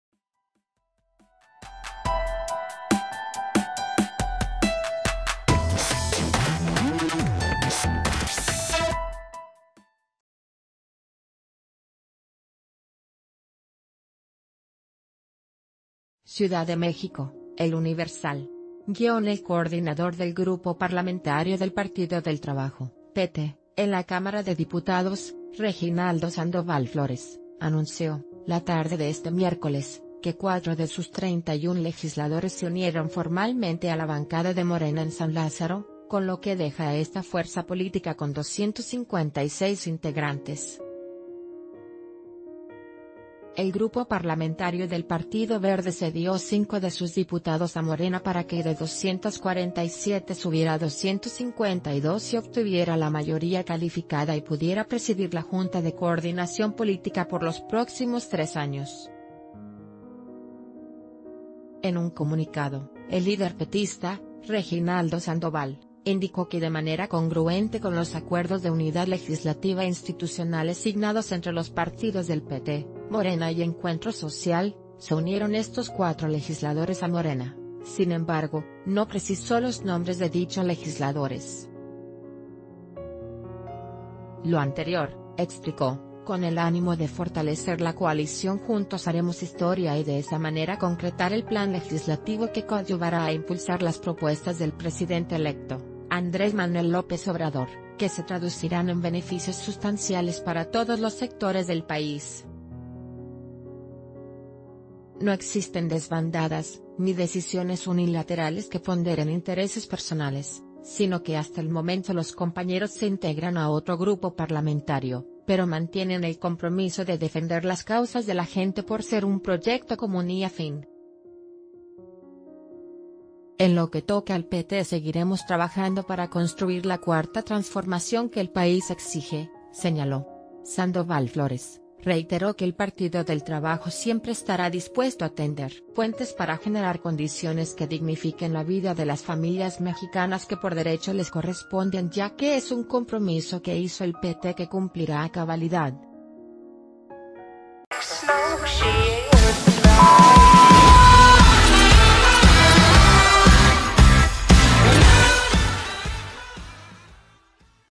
Con 256 integrantes, Morena es mayoría absoluta en San Lázaro - El Diario de Yucatán
CIUDAD DE MÉXICO (El Universal).- El coordinador del grupo parlamentario del Partido del Trabajo (PT) en la Cámara de Diputados, Reginaldo Sandoval Flores, anunció, la tarde de este miércoles, que cuatro de sus 31 legisladores se unieron formalmente a la bancada de Morena en San Lázaro, con lo que d...